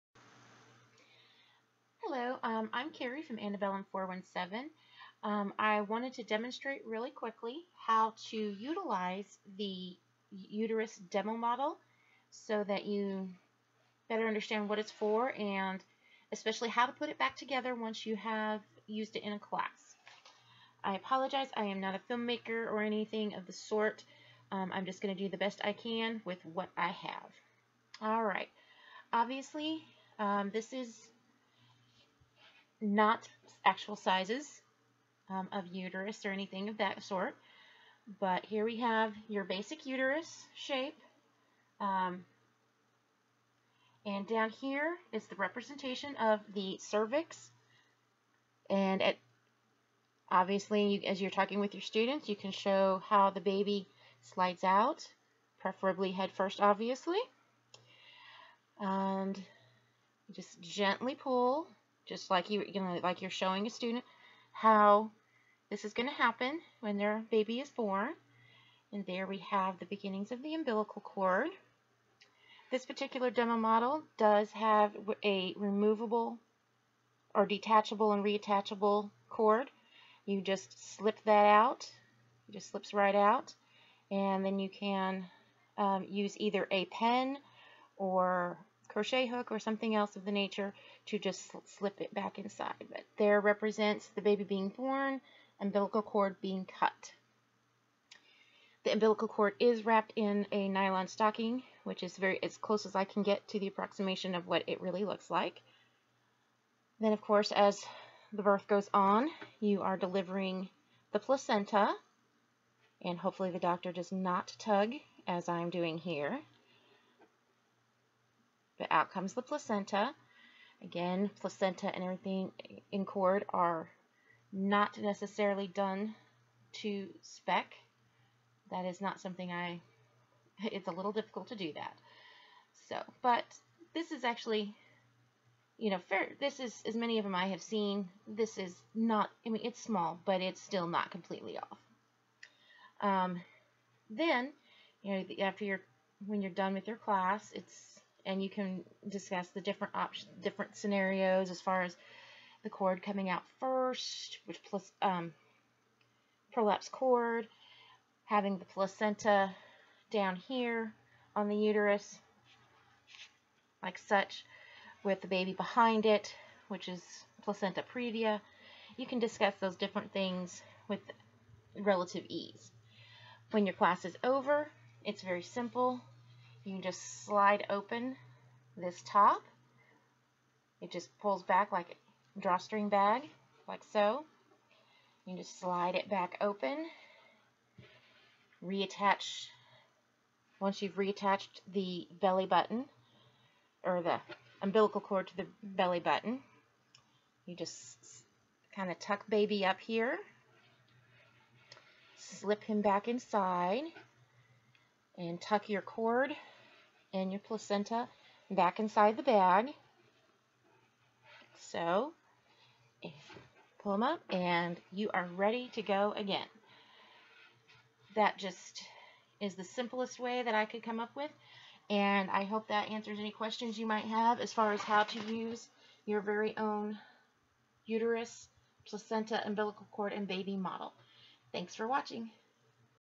Uterus, Placenta, Umbilical Cord & Baby Demonstration Model
This is a demonstration of how to use the demo model available for purchase
particularly how to reload the model for the next time.